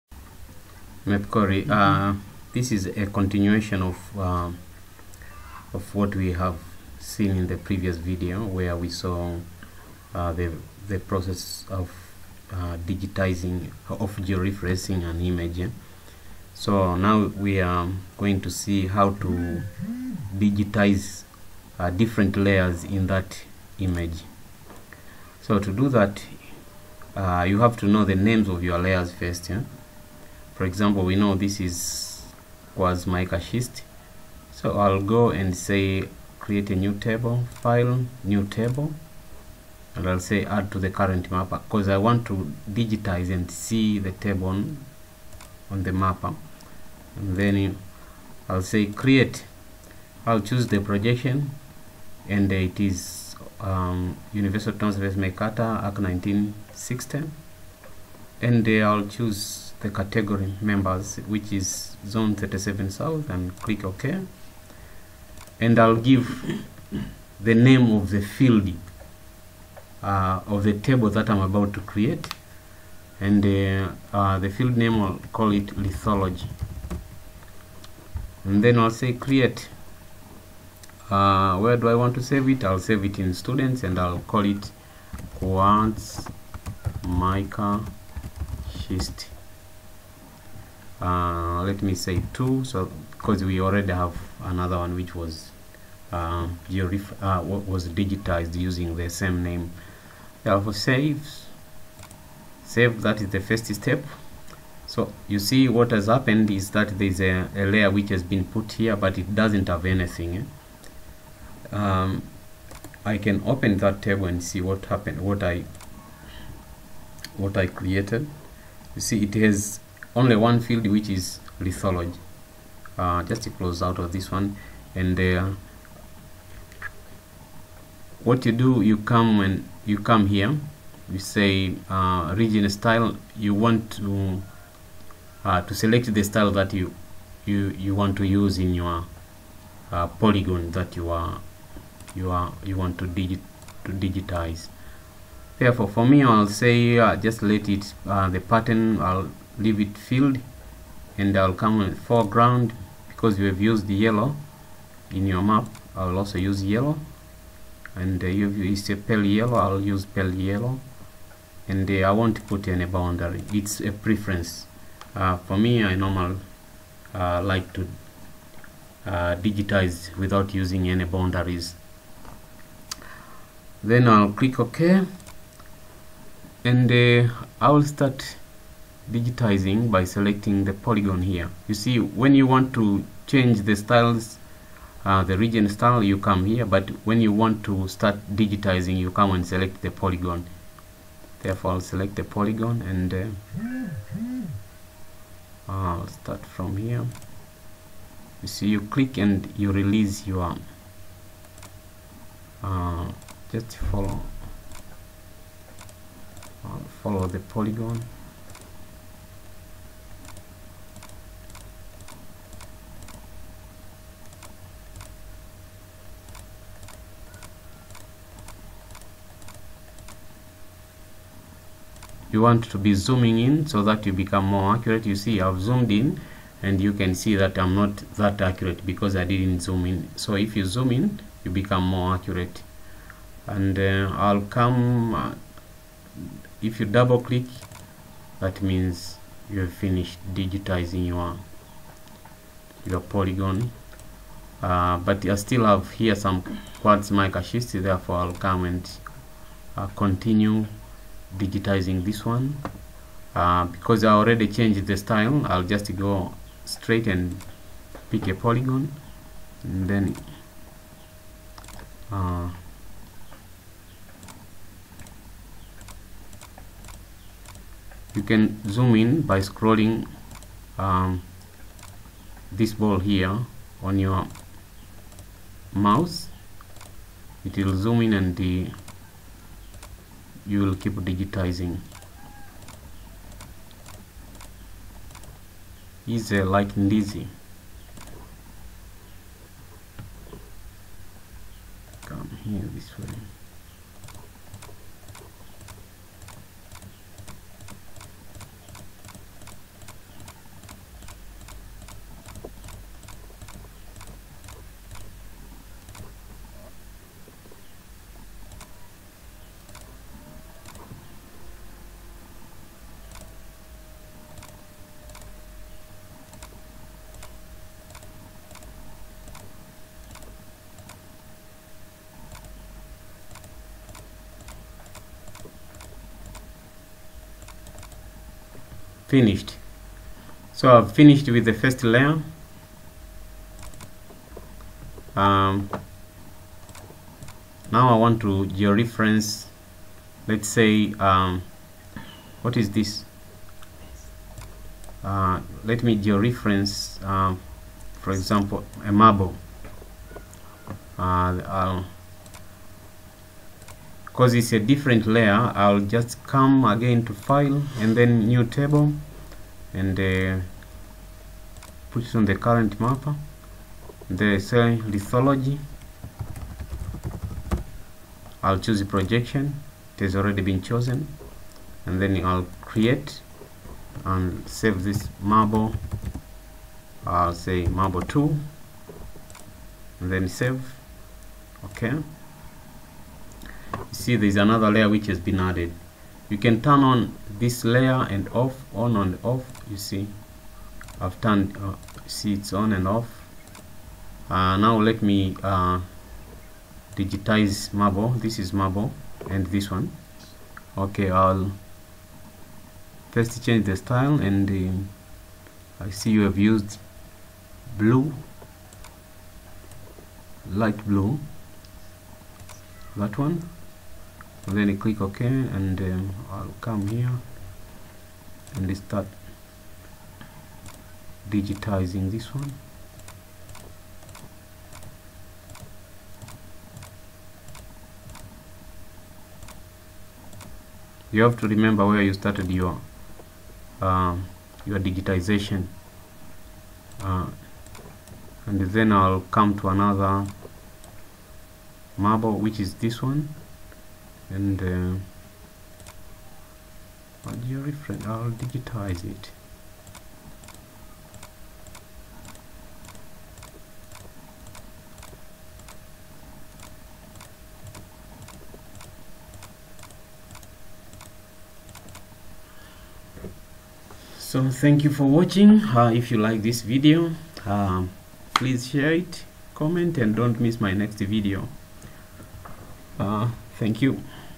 MapInfo How to digitize different layers in mapinfo using the geo referenced image
المصدر:
قناة  update your skill